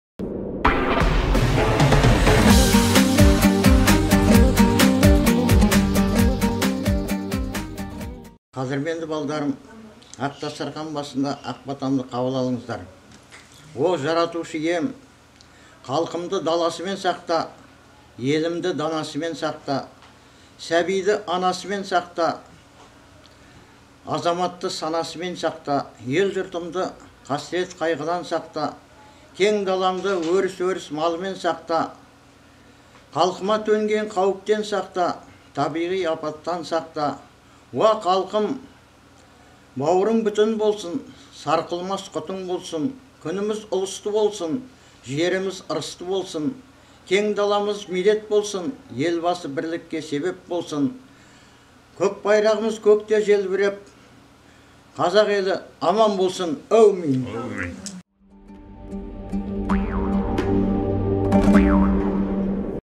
"Аталар батасы"
«Батаменен ел көгерер, жаңбырменен жер көгерер» деген халқымыз ежелден әрбір ісін батасыз бастамайтын. Ұрпағына үлкеннен бата алуды, елдің тілегін, ықыласын алуды санасына сіңіріп отырған.
         Бата - адал ниет, шынайы тілек білдіру дәстүрі. Сондай-ақ бата - қазақ халқының дінімен біте қайнасқан дәстүрінің көрінісі. Батаны көпті көрген, ортасына сыйлы, аузы дуалы, батагөй қариялар береді.
⚜⚜⚜Жамандықты қайтарып, жақсылықты шақыратын Қазақтың ғасырлар бойы бойына сіңген жақсы ырымы- бата алу ырымы.
  Назарларыңызға "АТА - ӘУЛЕТТІҢ  ТІРЕГІ, ӘЖЕ -ҰРПАҚТЫҢ ЖҮРЕГІ" атты аудандық байқауына қатысқан  қарияларымыздың  Наурыз баталарын ұсынамыз.